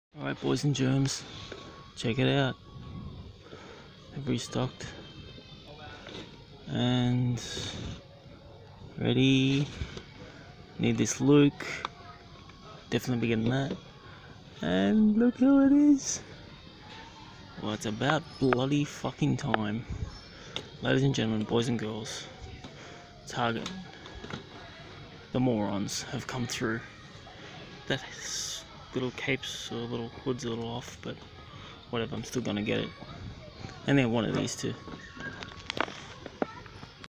Found! Black Series Dark Rey.
Star Wars Toy Run 0981977: found a Dark Rey, Star Wars Black Series The Rise Of Skywalker Rey for $35. over priced.